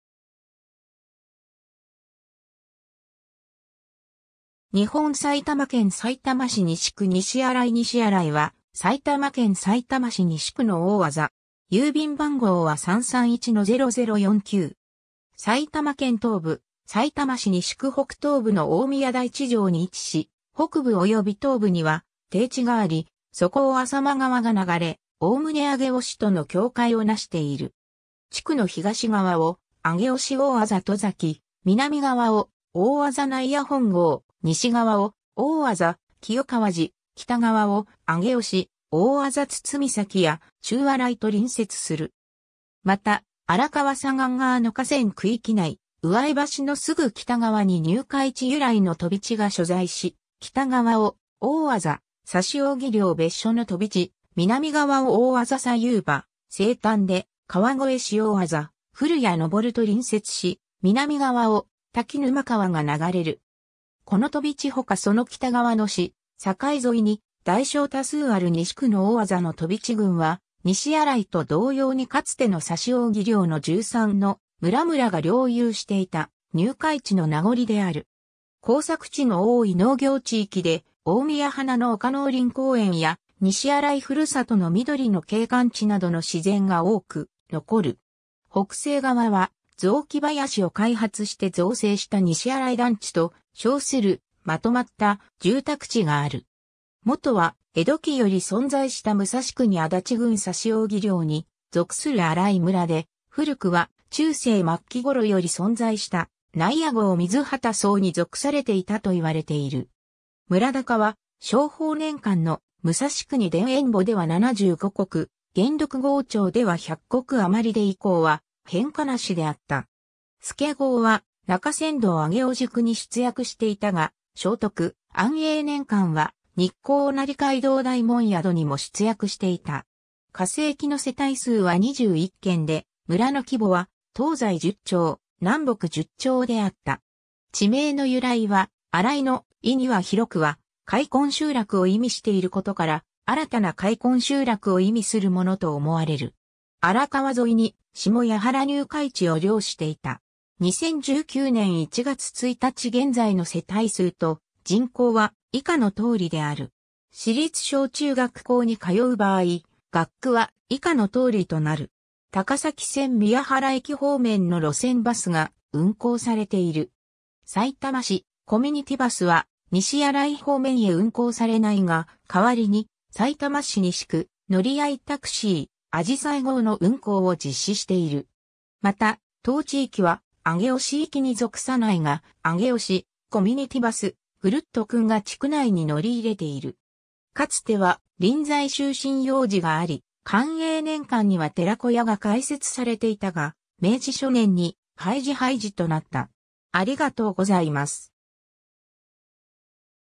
西新井 (さいたま市)
さいたま市西区の町・字
日本 」 埼玉県 」 さいたま市 」 西区 (さいたま市) 」 西新井 (さいたま市) 西新井（にしあらい）は、埼玉県さいたま市西区の大字。
 埼玉県東部、さいたま市西区北東部の大宮台地上に位置し、北部および東部には低地があり、そこを浅間川が流れ、概ね上尾市との境界を成している。
地区の東側を上尾市大字戸崎、南側を大字内野本郷、西側を大字清河寺、北側を上尾市大字堤崎や中新井と隣接する。
また、荒川左岸側の河川区域内、上江橋のすぐ北側に入会地由来の飛地が所在し、北側を大字指扇領別所の飛地、南側を大字西遊馬、西端で川越市大字古谷上と隣接し、南側を滝沼川が流れる。
この飛地ほかその北側の市境沿いに大小多数ある西区の大字の飛地群は、西新井と同様にかつての指扇領の13の村々が領有していた入会地の名残である。
 耕作地の多い農業地域で、大宮花の丘農林公苑や「西新井ふるさとの緑の景観地」などの自然が多く残る。
北西側は雑木林を開発して造成した西新井団地と称する纏まった住宅地がある。
 もとは江戸期より存在した武蔵国足立郡指扇領に属する新井村で、古くは中世末期頃より存在した内野郷水判土荘（みずはたのしょう）に属されていたと云われている。
村高は正保年間の『武蔵国田園簿』では75石（田33町余、畑41町余、野銭永1貫230文）、『元禄郷帳』では100石余で以降は変化なしであった。
助郷は中山道上尾宿に出役していたが、正徳・安永年間は日光御成街道大門宿にも出役していた。
化政期の世帯数は21軒で、村の規模は東西10町、南北10町であった。
地名の由来は新井の「井（イ）」には広くは開墾集落を意味していることから、新たな開墾集落を意味するものと思われる。
荒川沿いに下谷原入会地を領していた。
 2019年（平成31年）1月1日現在の世帯数と人口は以下の通りである。
 高崎線宮原駅方面の路線バスが運行されている。
さいたま市コミュニティバスは西新井方面へ運行されないが、替わりにさいたま市西区乗合タクシー「あじさい号」の運行を実施している。
また、当地域は上尾市域に属さないが、上尾市コミュニティバス「ぐるっとくん」が地区内に乗り入れている。
 かつては臨済宗新養寺があり、寛永年間には寺子屋が開設されていたが、明治初年に廃寺廃寺となった。